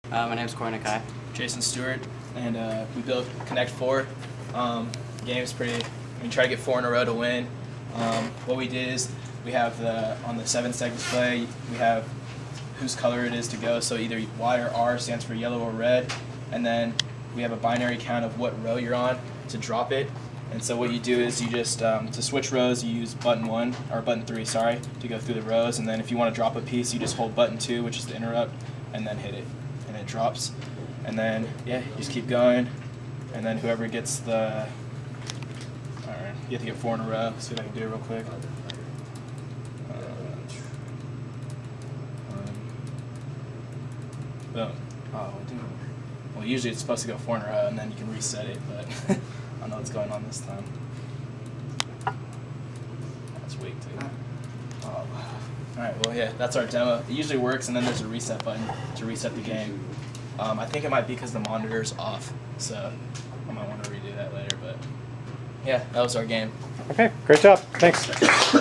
Connect 4
Nexys2 Board projects, done by Calpoly SLO students in their Sophomore year digital circuits class (CPE-269-01, Fall 2009).

Nakai, Corey H.
Stewart, Jason A.